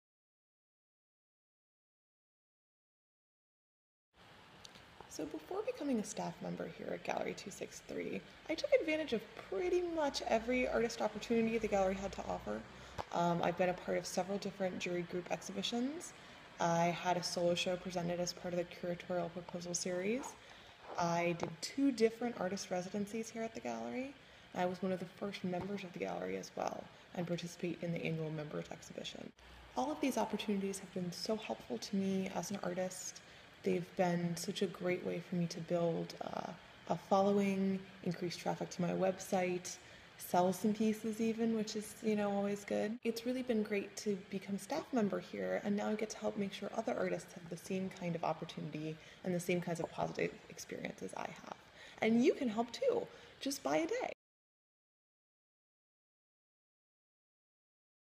Alexa on Artist Opportunities at Gallery 263
Our new Managing Director, Alexandra Photopoulos, talks about showing her work at Gallery 263, becoming an Artist Member, and her experience as a Resident Artist.
Support 365 Days of Gallery 263 by sponsoring a day! #365/263